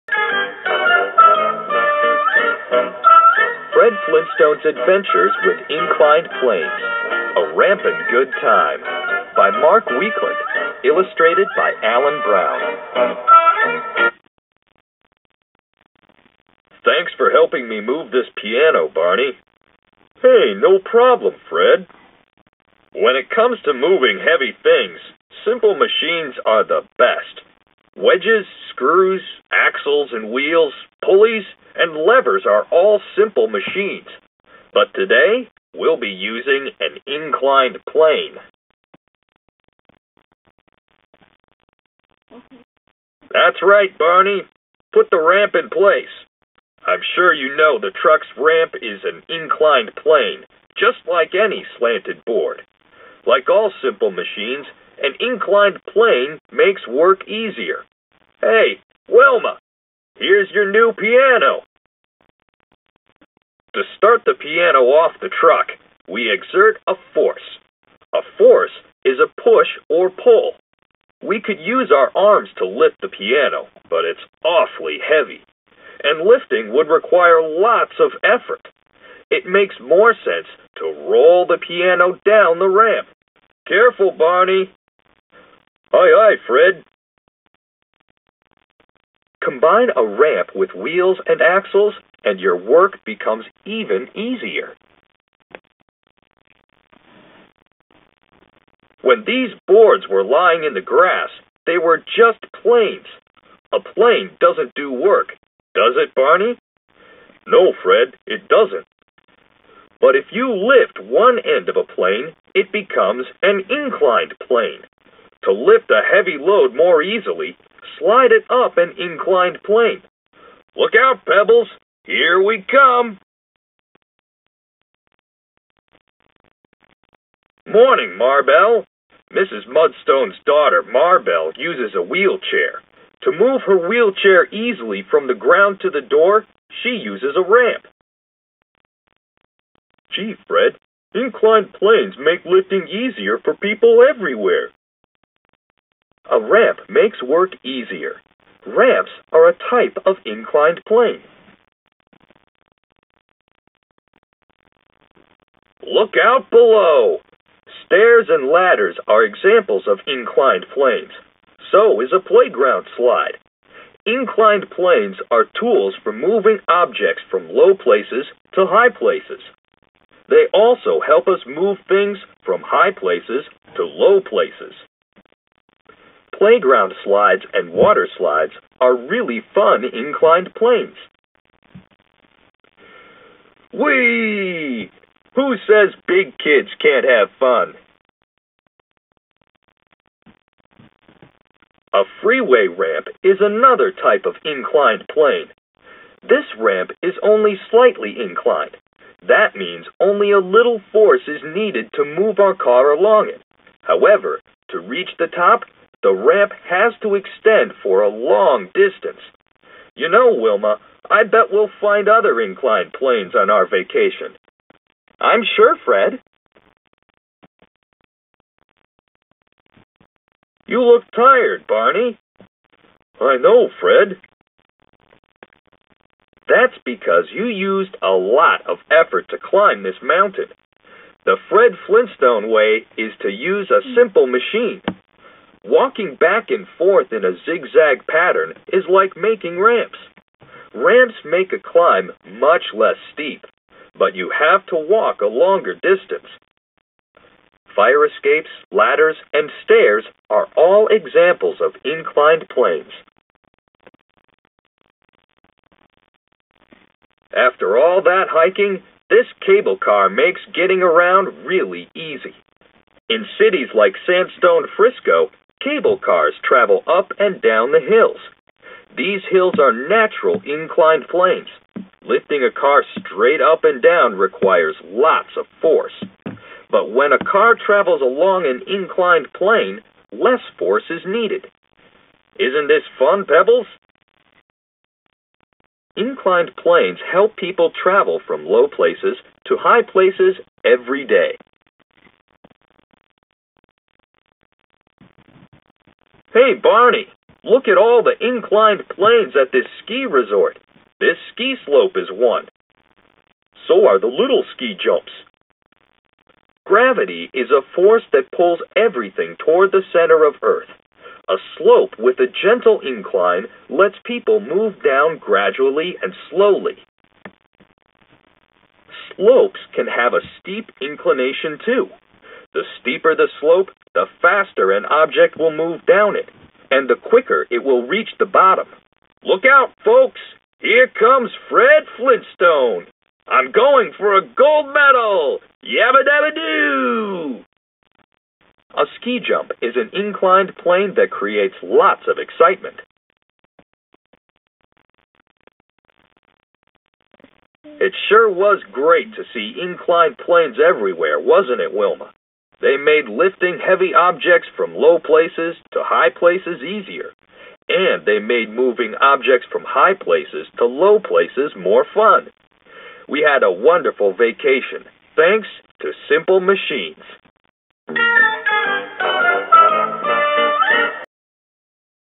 Fred Flintstone's Adventures with Inclined Planes
Have a Yabba-Dabba-Doo day with Fred Flintstone as he explains how an inclined plane works! Fred Shows readers the Flintstone way of using simple machines in everyday situations. With simple,informative  text narrated by Fred and a few other Flintstone characters paired with colorful illustrations, students will learn all about simple machines and basic physics concepts!

Recommended Grade Levels: KG-2
                                 Language : English
                                       Author : Weakland, Mark
                                  Illustrator : Brown,Alan
                                   Publisher: Capstone Press
                               Book series: Flintstone explain simple machines
                                          Lexile: 890L